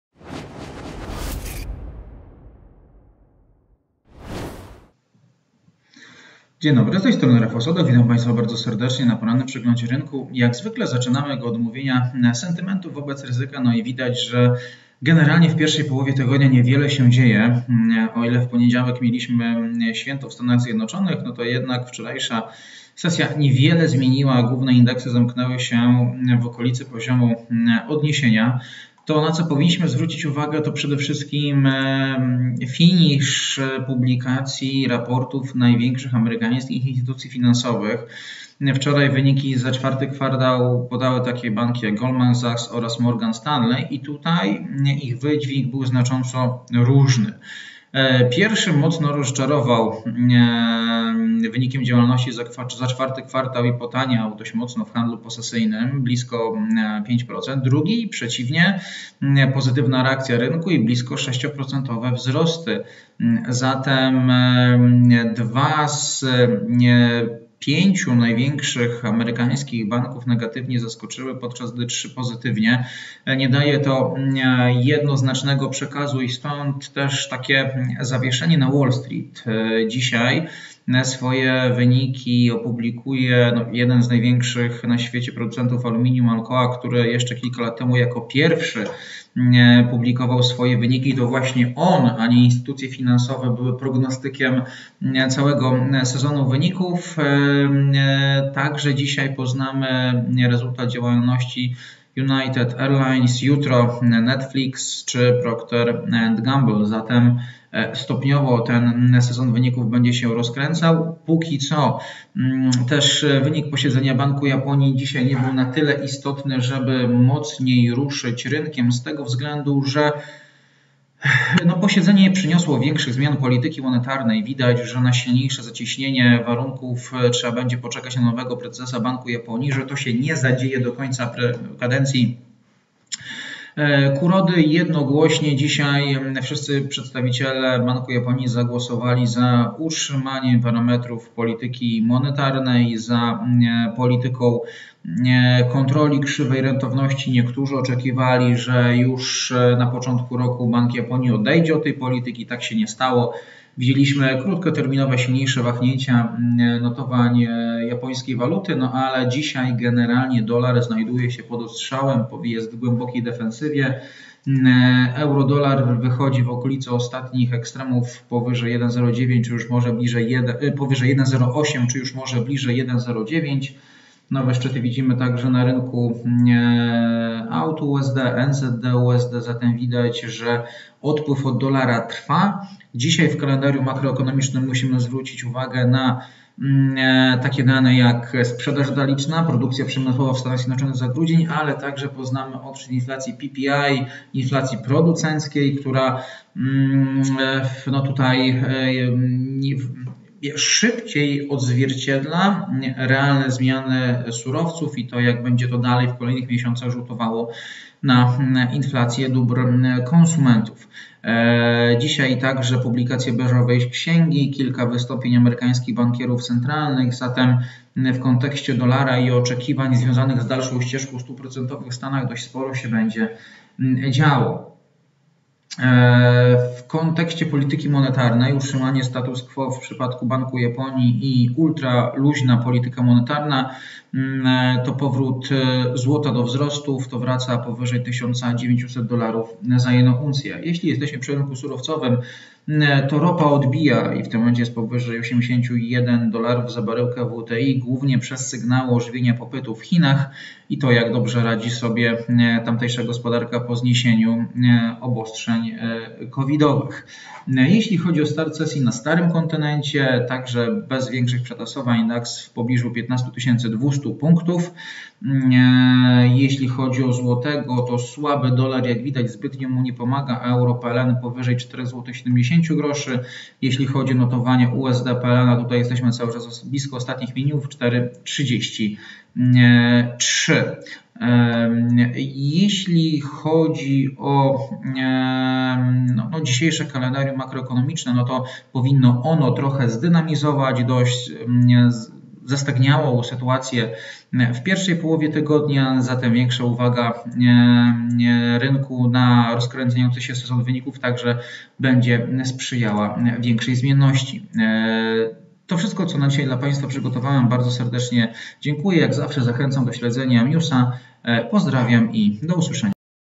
Analiza poranna – Bank Japonii nie zaskoczył 18.01.2023
Wtorkowa sesja na Wall Street nie przyniosła większych przetasowań, a główne indeksy zamknęły się w pobliżu poziomu odniesienia. Środowy poranek również nie przynosi większej zmienności, a nastroje pozostają stabilne bowiem notowania kontraktu terminowego na S&P500 są cały czas w pobliżu 4000 pkt. Nie inaczej wygląda to w przypadku niemieckiego Daxa, który znajduje się nieco powyżej 15200 pkt. Dziś uwaga rynków skupiała się na posiedzeniu Banku Japonii, który jednak utrzymał parametry polityki monetarnej na niezmienionym poziomie, co negatywnie wpłynęło na kurs jena i wsparło giełdę w Tokio. Podbiło także notowania złota, które wróciły powyżej 1900 USD za uncję. Poza tym nie było widać szerszych konsekwencji.